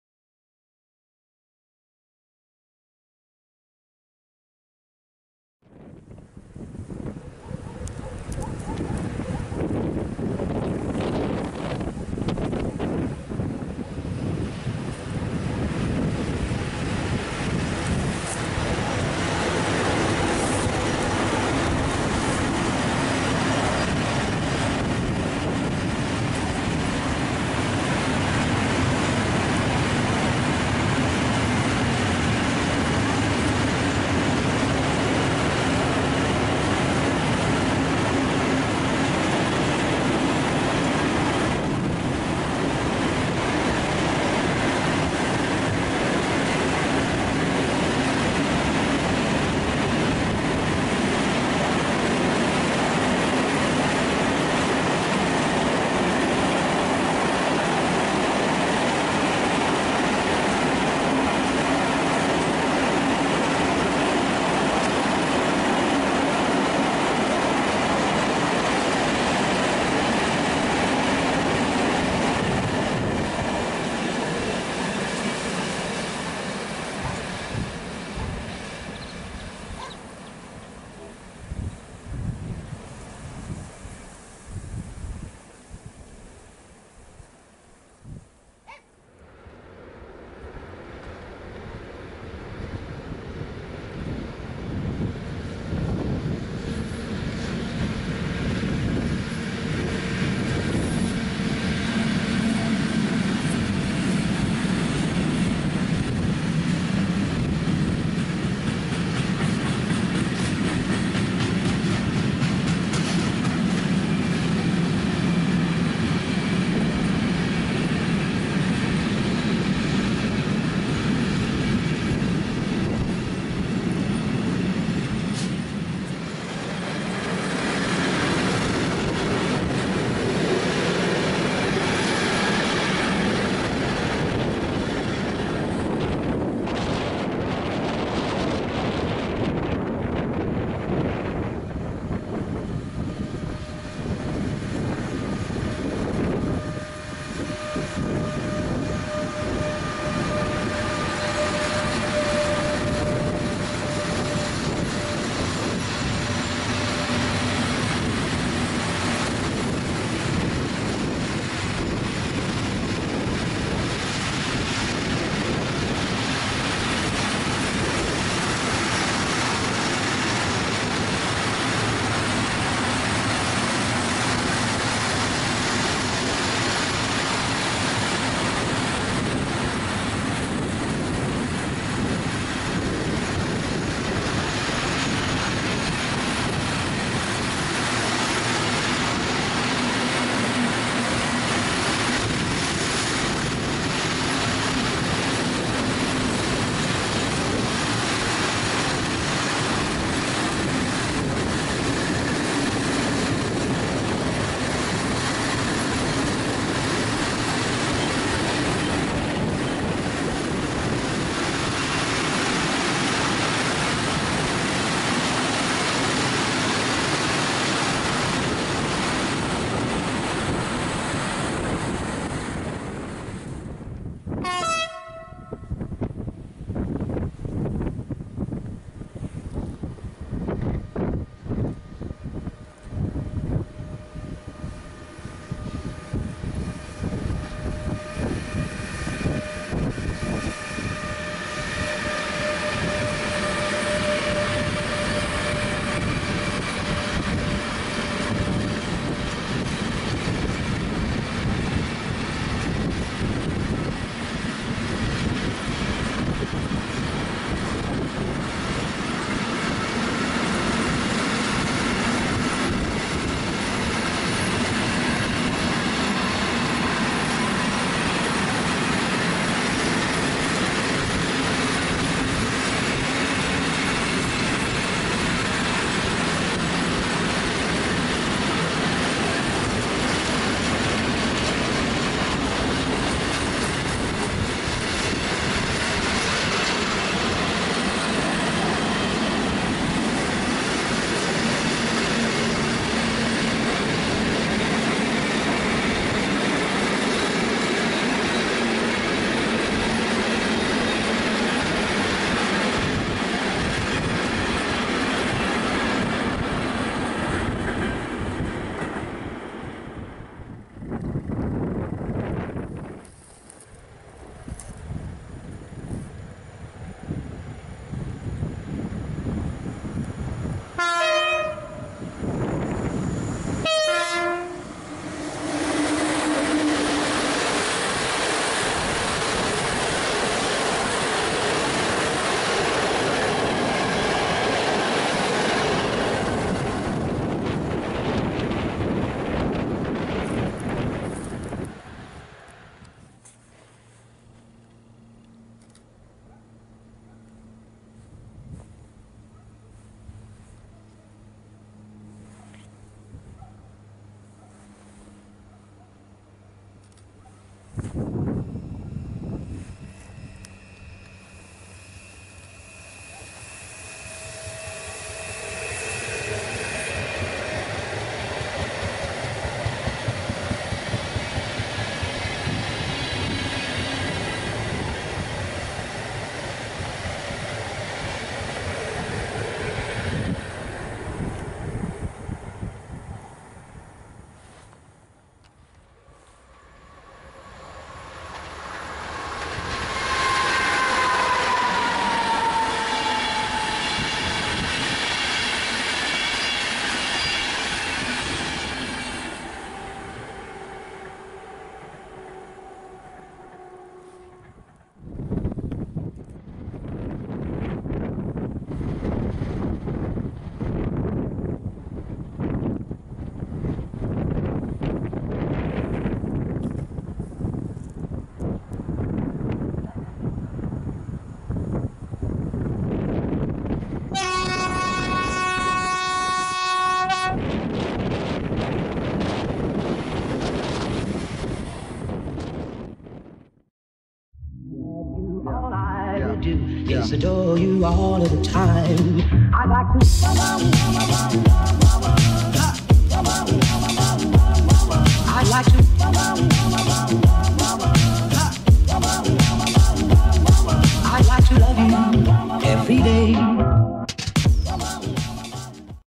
Kutno Azory 17.03.18
Zapraszam Do Oglądanie składanki z pociągów/składów nagranych dnia 17.03.18

Pozdrawiam Maszynistów! :)

Nagrane Składy:
1. CTL Logistics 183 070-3 ze składem węglarek wyjeżdża z Kutna i podąża wstronę Barłogi i podjazd Traxx,a (PCC Intermodal) E186 #?
2. Lotos EU07-199 ze składem kontenerów podąża w stronę Włocławek, a od razu za nim wyjeżdża z placu Traxx Lotos Kolej E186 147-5
3. PCC Intermodal Traxx E186 #? Ze składem kontenerów wjeżdża na stacje Kutno Azory
4. PKP Intercity EU07-379 TLK wyjeżdża z Kutna w stronę z Włocławek a zaraz przed nim TLK ciągnięte przez EU07-368
5. PKP Intercity ED160-019 z Włocławek wjeżdża na stacje Kutno Azory.
6. EP09-026 PKP Intercity (Niewiem skąd) Zazwyczaj na tej linii jeżdża różowe Taurusy PKPIC.
7. Koleje Mazowieckie SA105-??? Podąża ze stacji Kutno Azory w stronę Płocka.